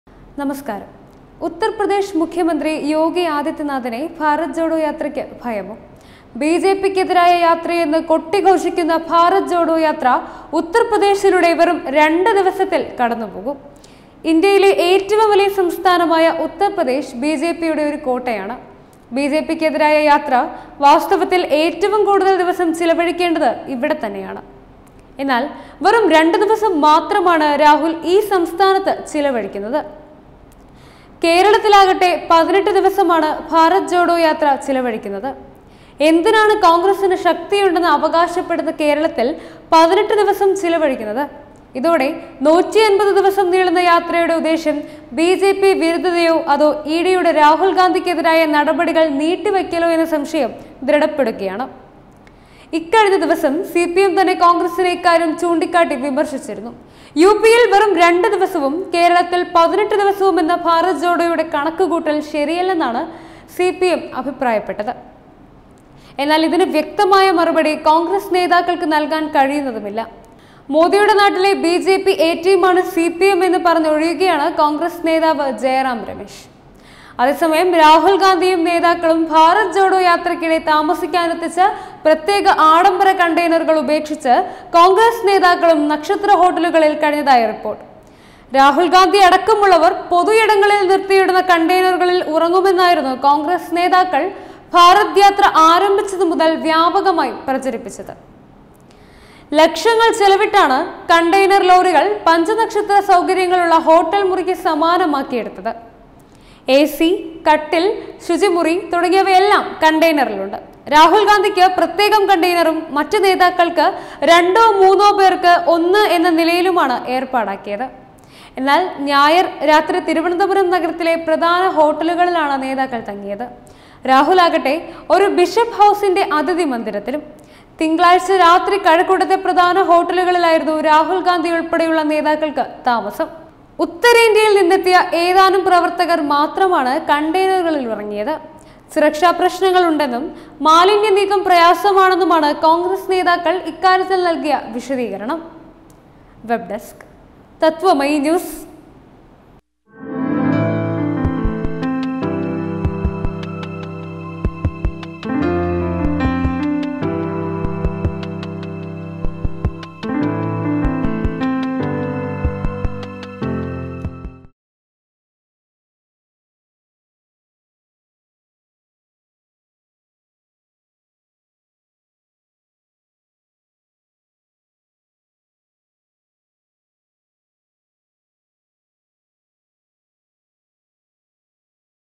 ജോഡോ യാത്രയുടെ ഉദ്ദേശ്യം ബിജെപി വിരുദ്ധതയോ ഇഡി നടപടികള്‍ നീട്ടിവെയ്ക്കലോ ?
ഇ ഡിയെ പേടി ? ജോഡോ യാത്രയുടെ യഥാർത്ഥ ഉദ്ദേശ്യം പുറത്ത്! ജോഡോ യാത്രയുടെ യഥാർത്ഥ ഉദ്ദേശ്യം ബിജെപി വിരുദ്ധതയോ അതോ രാഹുല്‍ഗാന്ധിയ്ക്കെതിരായ ഇഡി  നടപടികള്‍ നീട്ടിവെയ്ക്കലോ ?